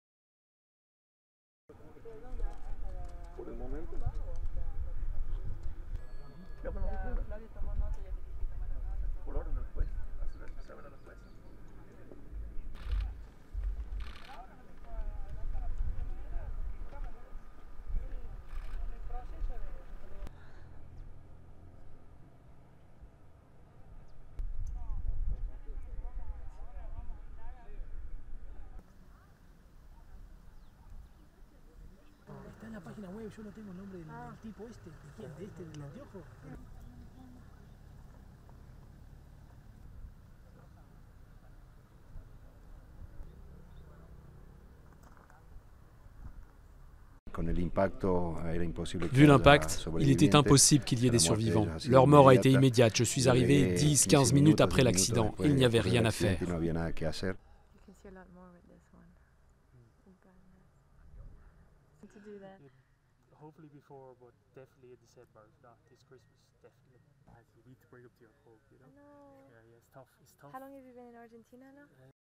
Crash en Argentine: des experts français attendus sur place
Des experts français du Bureau enquête analyse (BEA) étaient attendus mercredi 11 mars à Villa Castelli, village argentin où deux hélicoptères se sont écrasés, tuant dix personnes dont trois grands sportifs français. Durée: 01:12